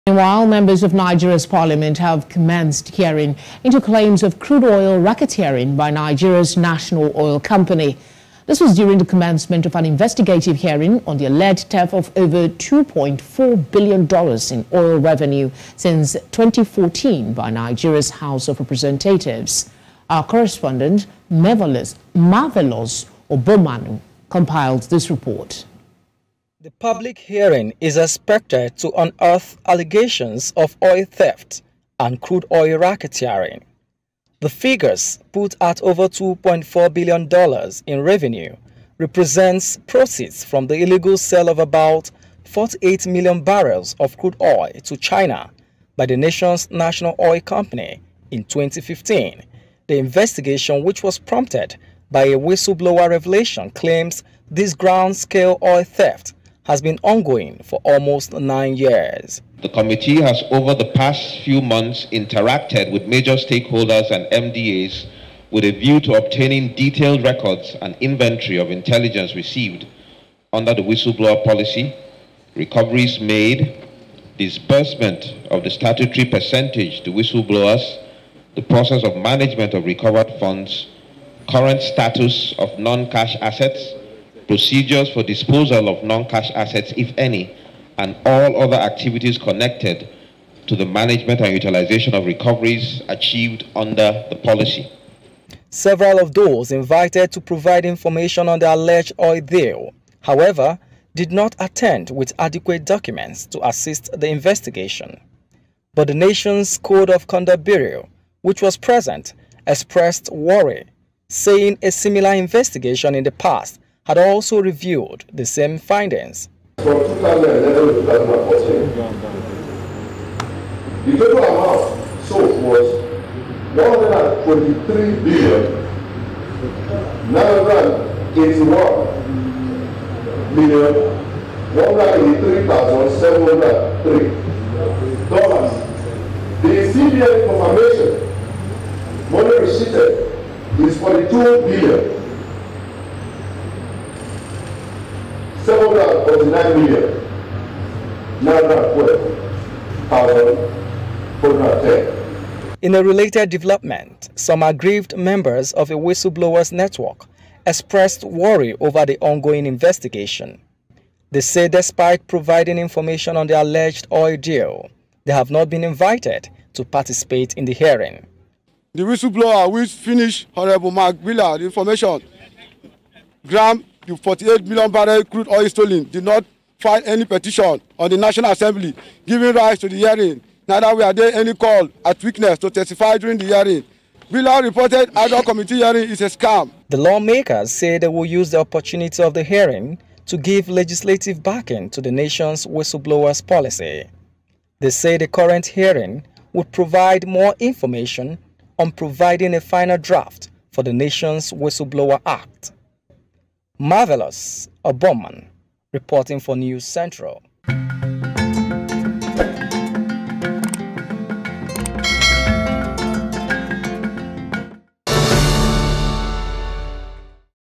Nigeria's House of Representatives Launch Investigation Hearing Over Oil Theft in the Country
Members of Nigeria’s Parliament have commenced hearing into claims of Crude Oil racketeering by Nigeria’s National oil company.
This was during the commencement of an investigative hearing on the alleged theft of over two point four billion dollars in oil revenue since twenty-fourteen by Nigeria’s House of Representatives.
Our Correspondent Marvelous Obomanu compiled this report.